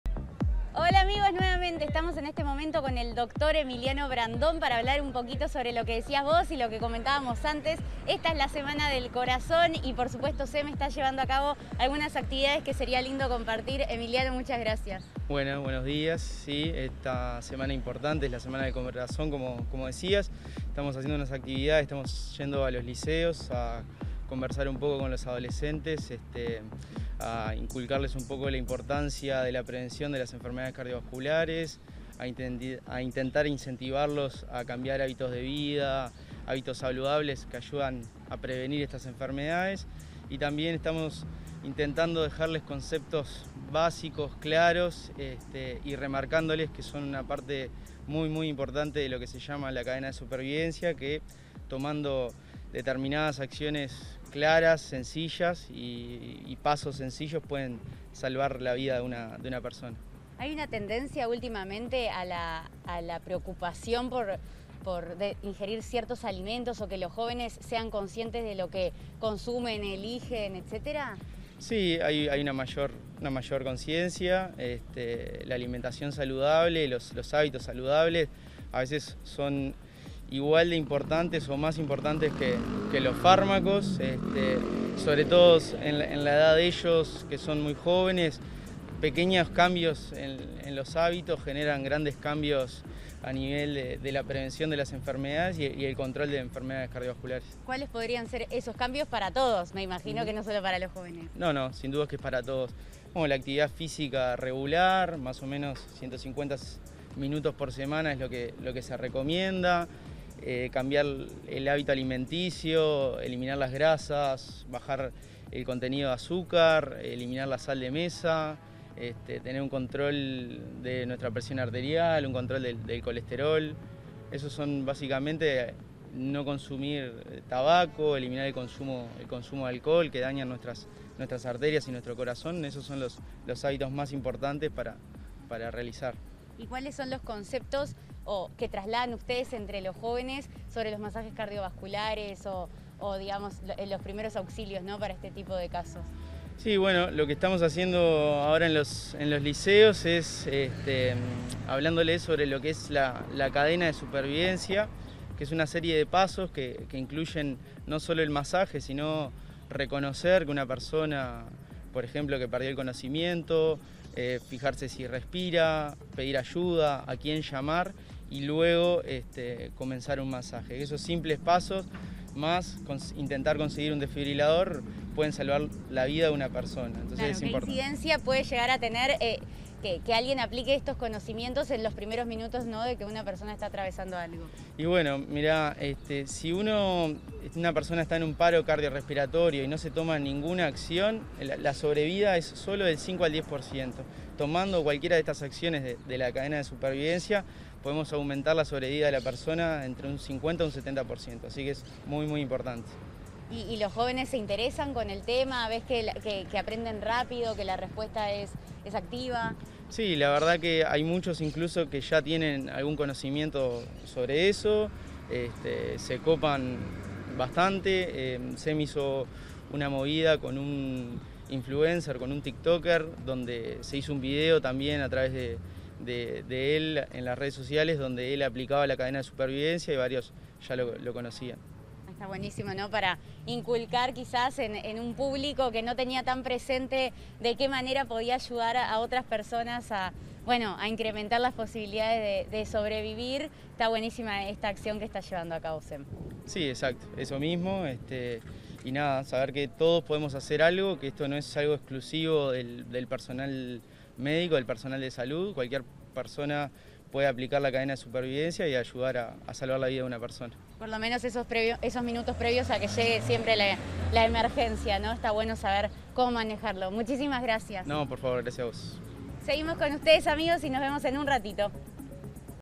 SEMM conmemora una nueva Semana del Corazón
La empresa SEMM conmemora esta nueva semana mediante la concientización sobre enfermedades cardiovasculares y la promoción de hábitos saludables que hagan a una mejor calidad de vida.